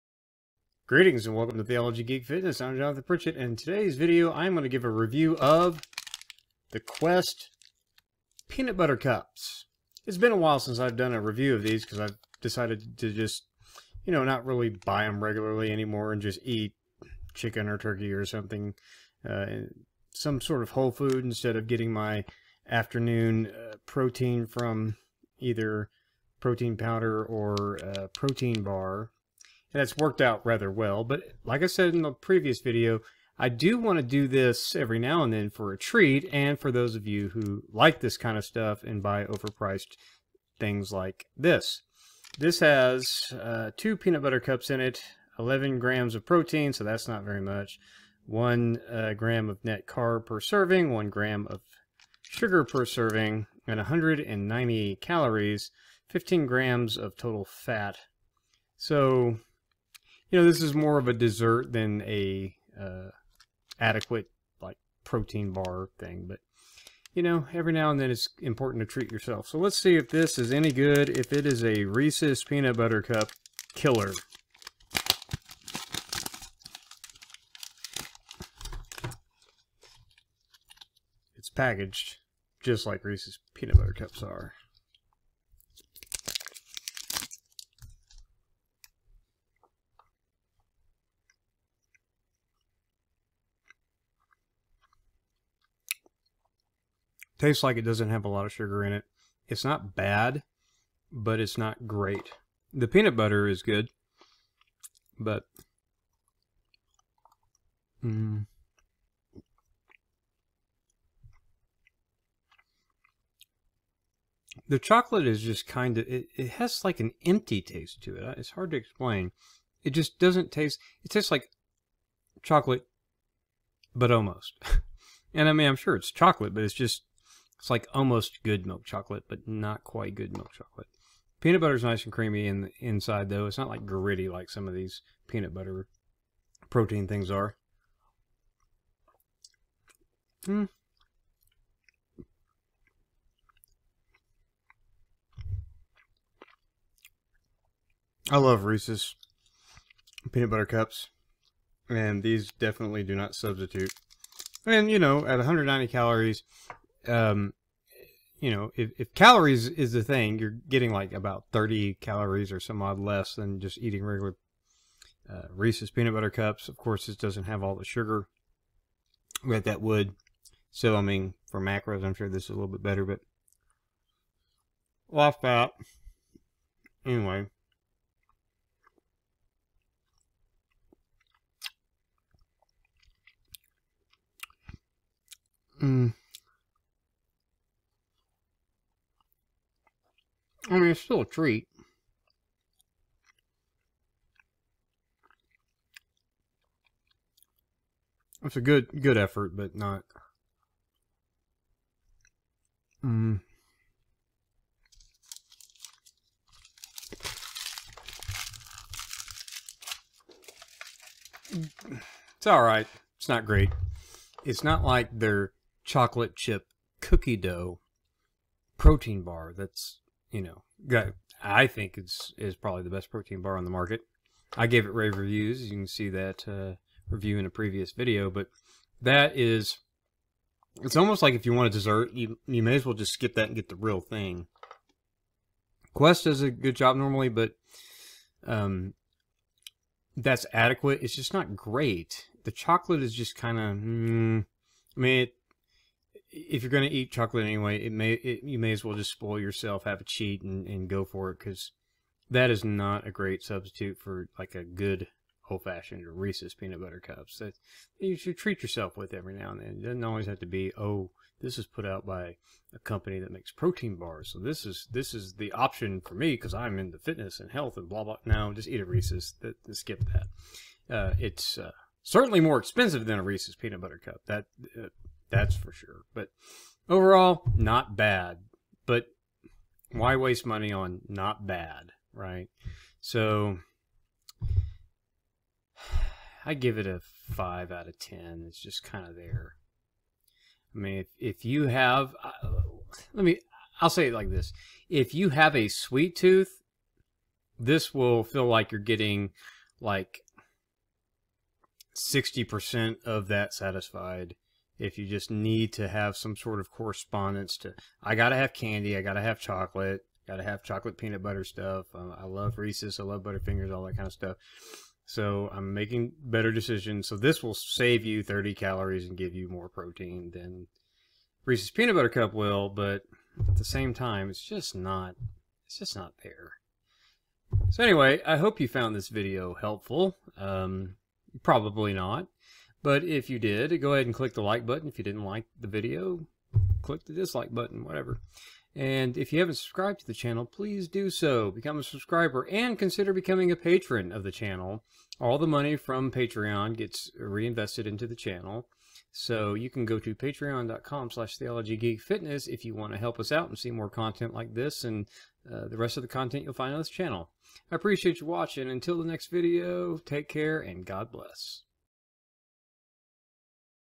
Quest Peanut Butter Cups Review
In this video, I discuss whether or not the Quest Peanut Butter Cups are a REESE's Peanut Butter and Chocolate Candy replacement.